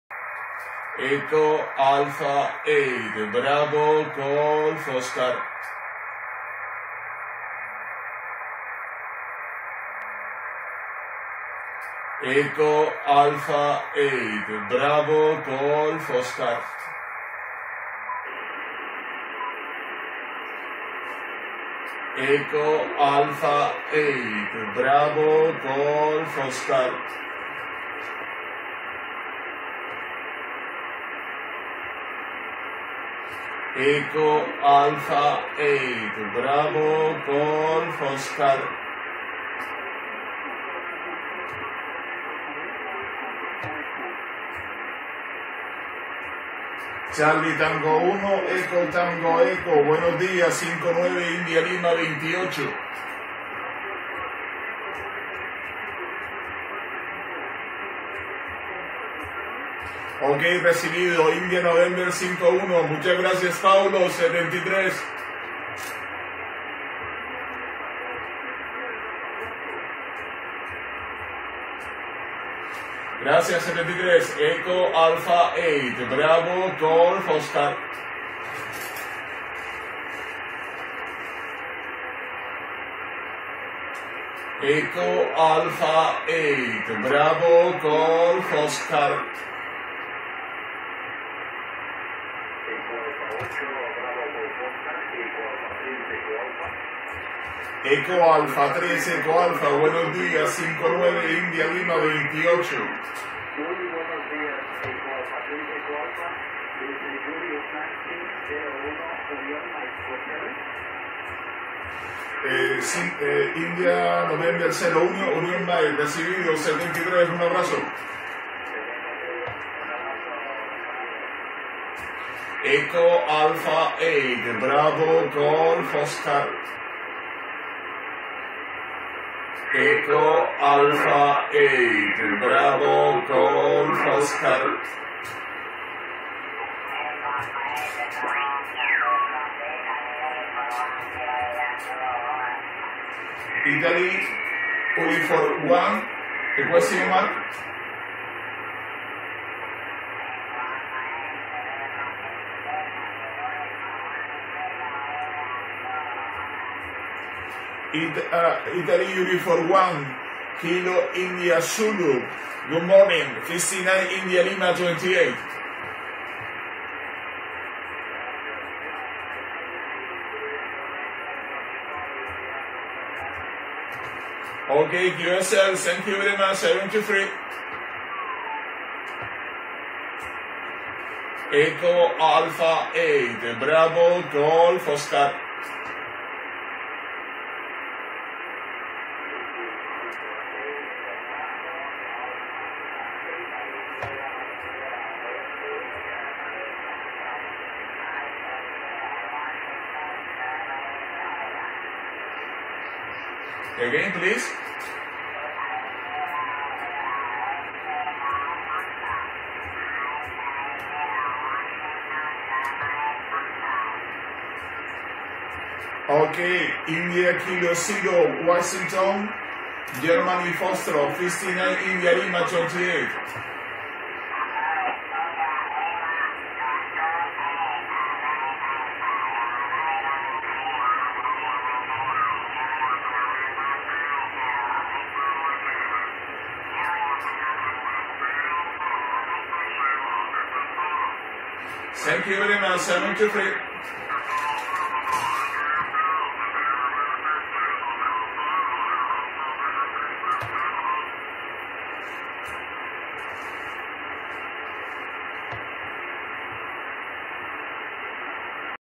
Contacts via amateur radio satellite RS-44
Contacts via amateur radio satellite RS-44 with CT1ETE, EA3EA, IU1KIZ and IK0WGF